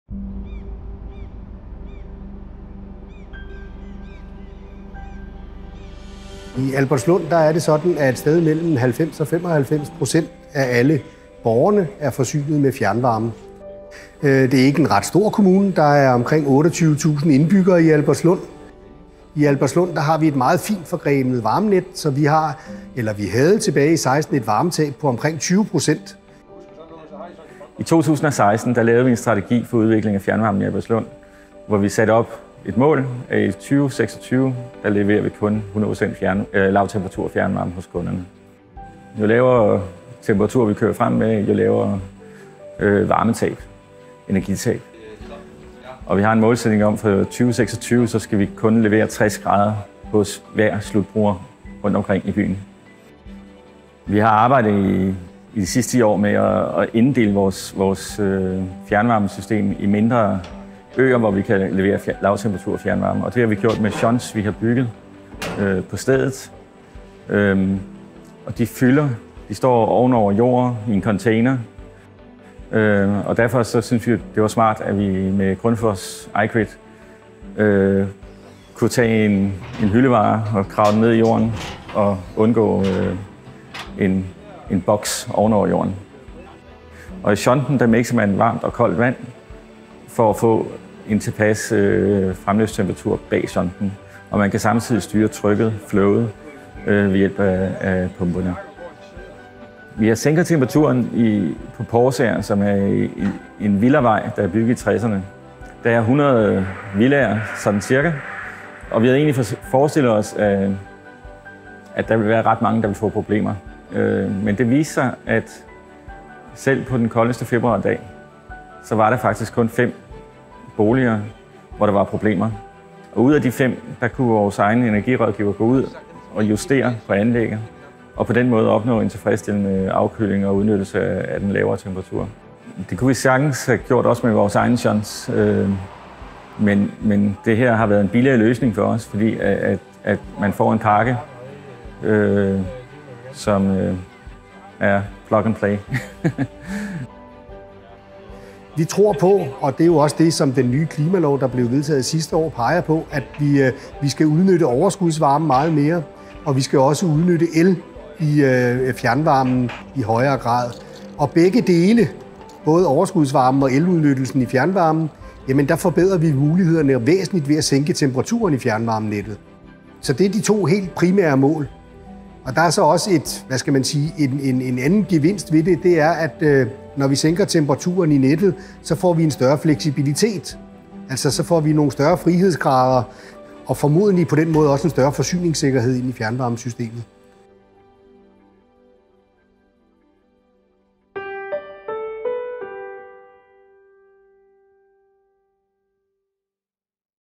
Albertslund enjoys ease of iGRID to control low-temp district heating
Albertslund Forsyning, a district heating utility for a suburb of Copenhagen, wants to reduce its district heating outward temperature to 60°C – from up to 100°C – in order to reduce heat loss in the pipes and save energy. After experimenting with its own above-ground low-temp shunts for years, it installed a submerged iGrid shunt/low temperature mixing station from Grundfos. Not only is it proving that it is a cost-beneficial solution for the low-temperature strategy, but the utility is also seeing the benefits of now being able to use renewable energy sources and surplus heat from buildings – plus making the overall system more efficient. See the case video or read the full story

This project has received funding from the European Union’s Horizon 2020 research and innovation programme under grant agreement N. 857811.